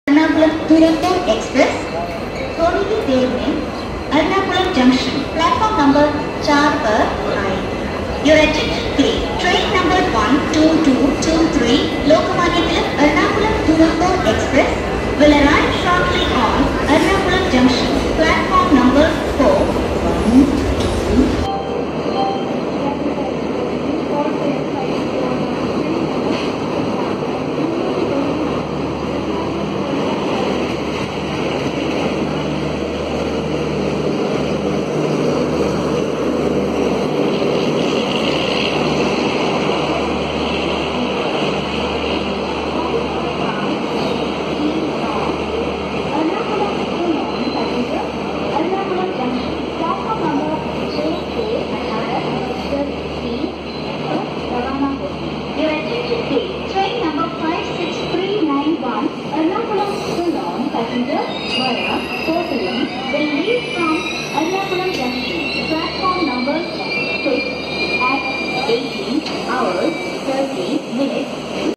Ernakulam Duronto arrives Ernakulam Junction
Gandhi livery Kalyan (KYN) WDM3D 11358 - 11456 paired 12223 Lokmanya Tilak Terminus - Ernakulam Junction Duronto Express arriving destination.